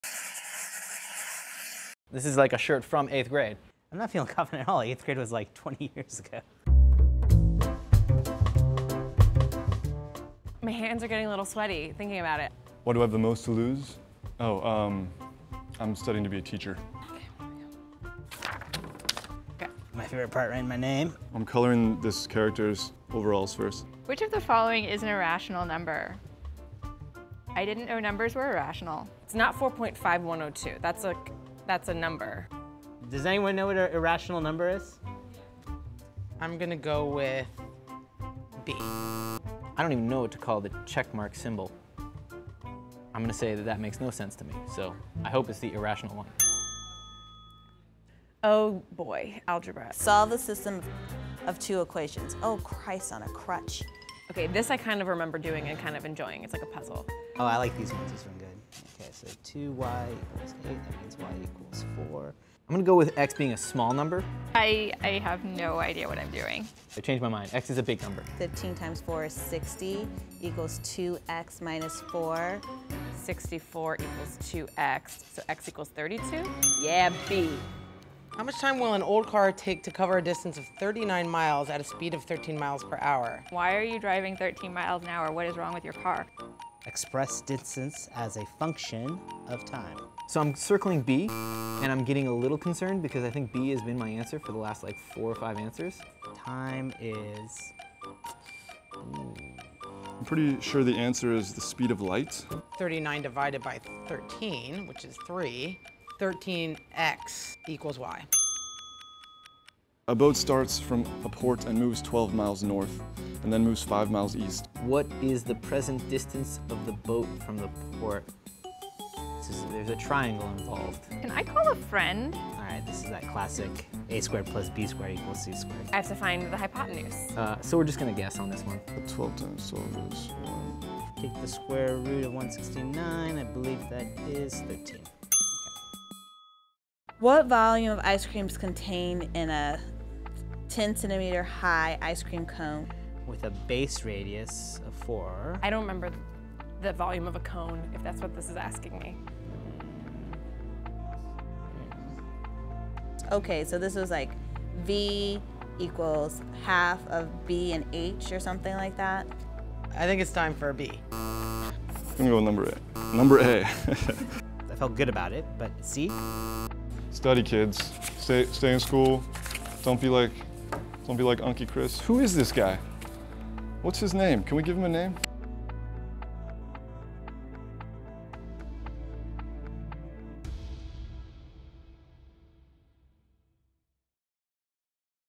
Adults Take 8th Grade Common Core Math Test
Common Core test standards that were implemented in 2010 have been controversial in schools across the nation. Algebra was hard the first time around, when we had to worry about puberty and pimples, too. But is it any easier as adults? AJ+ staff takes a Common Core 8th grade math test to find out.